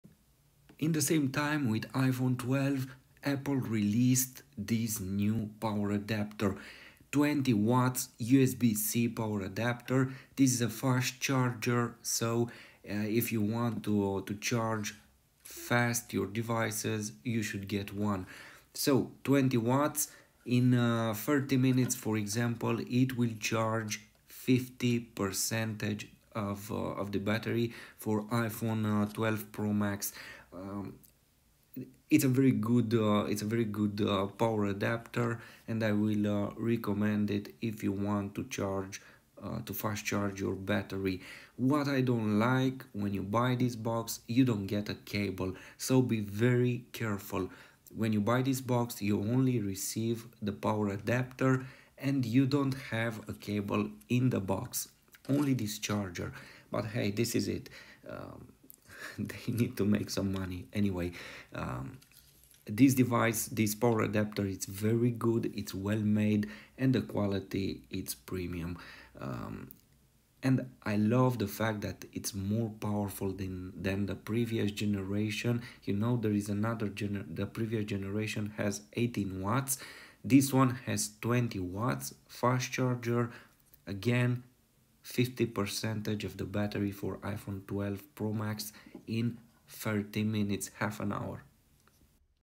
Unboxing the New Apple fast charger 20W USB‑C - Power Adapter 2020
The New Apple fast charger 20W USB‑C - Power Adapter released in 2020 - genuine product bought from Apple store.